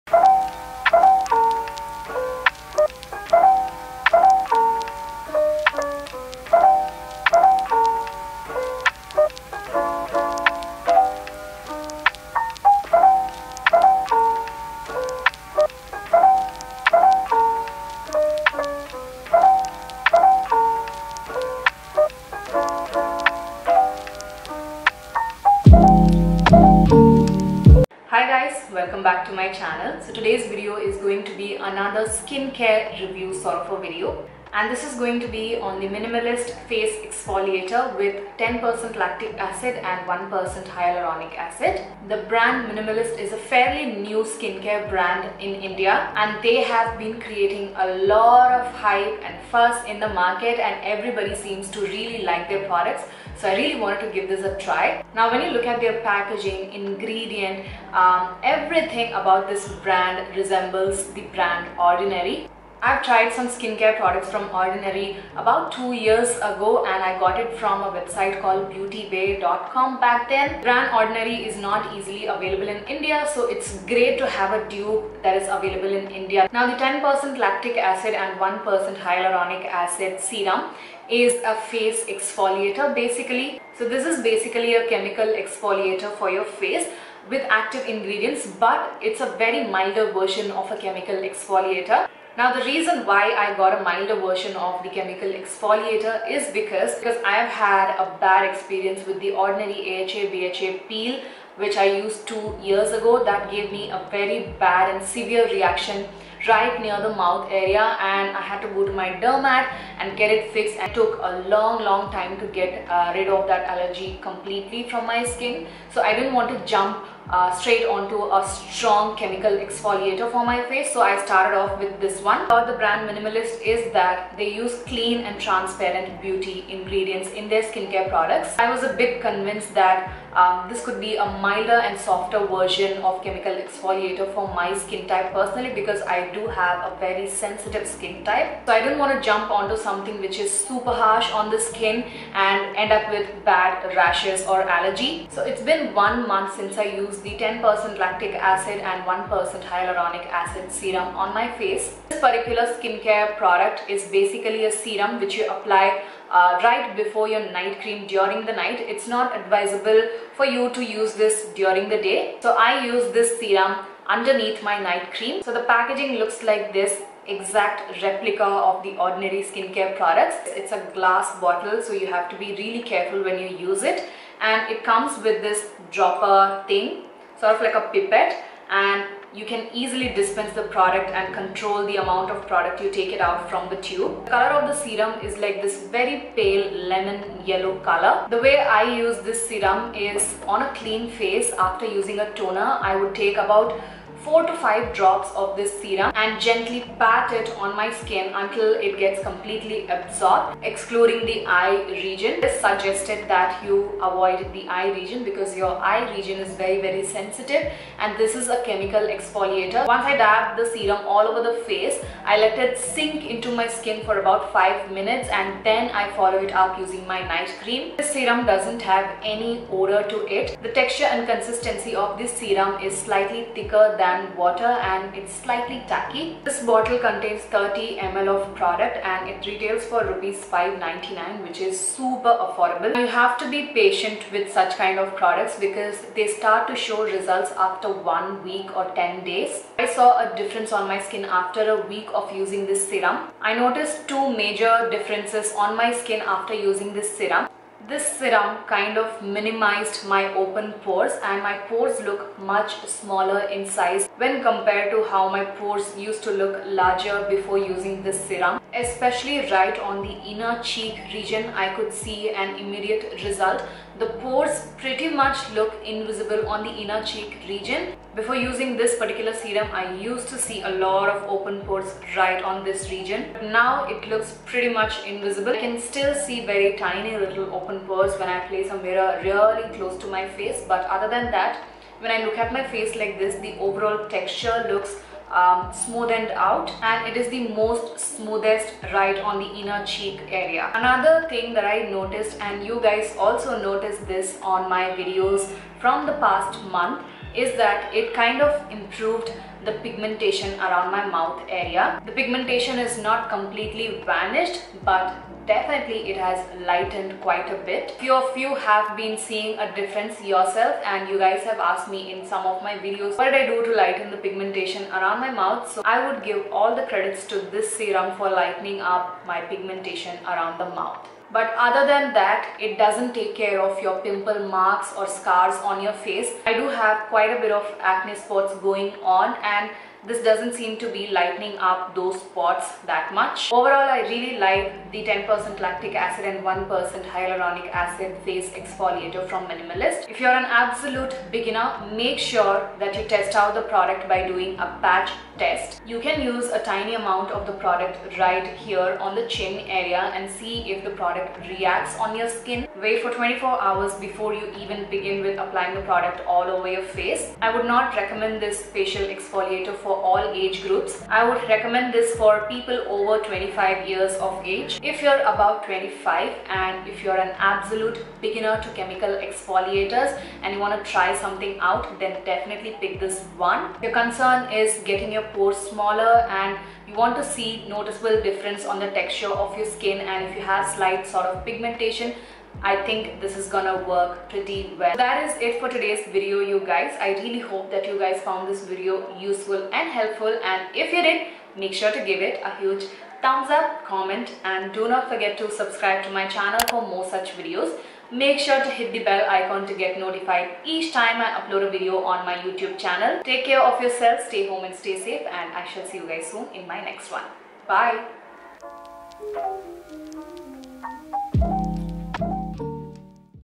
Minimalist Lactic Acid 10% + 1% Hyaluronic Acid | Results after using for One Month
Hey everyone , in today’s video I have shared my review on the Minimalist 10% Lactic Acid + 1% Hyaluronic Acid exfoliating serum after using it for a month.

PRIYA20

Applicable on minimum purchase of Rs.1000

PRODUCTS USED:

Minimalist 10% Lactic Acid + 1% Hyaluronic Acid exfoliating serum

Limited-time deal: Minimalist Lactic Acid (AHA) 10% Exfoliant for Acne Scars, Even Tone & Texture | Facial Exfoliating Serum with Hyaluronic Acid for Dry, Oily or Sensitive Skin | 30ml